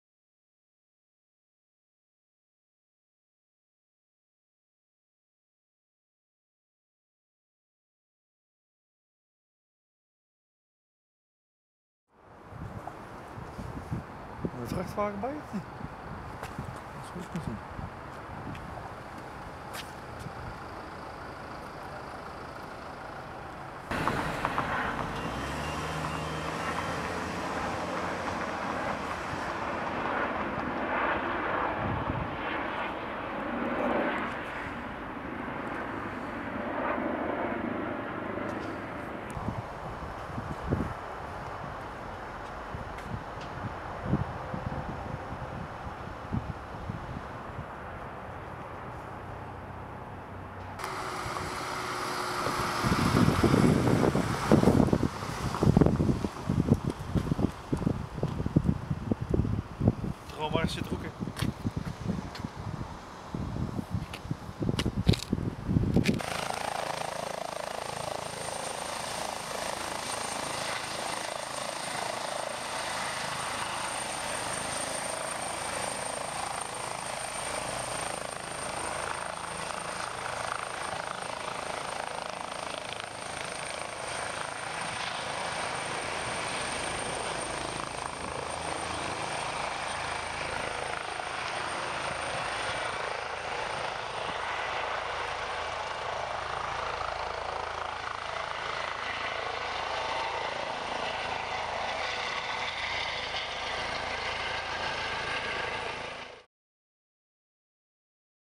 (VIDEO) 19-02-2015 Traumahelikopter landt in Oosterhout, man zwaargewond bij bedrijfsongeval
OOSTERHOUT – Bij een bedrijfsongeval bij koekjesfabriek Merba aan het Wilhelminakanaal noord in Oosterhout is donderdagavond een persoon in zorgwekkende toestand naar het ziekenhuis gebracht.

Meerdere ambulances en een traumahelikopter waren ter plaatse. De man is zwaargewond naar het ziekenhuis in Tilburg gebracht. Een trauma-arts is met de ambulance meegereden. Het slachtoffer zou tijdens het lossen van zijn vrachtwagen zijn gevallen.

De toedracht wordt onderzocht, de technische recherche en de arbeidsinspectie komen ter plaatse om de toedracht te onderzoeken.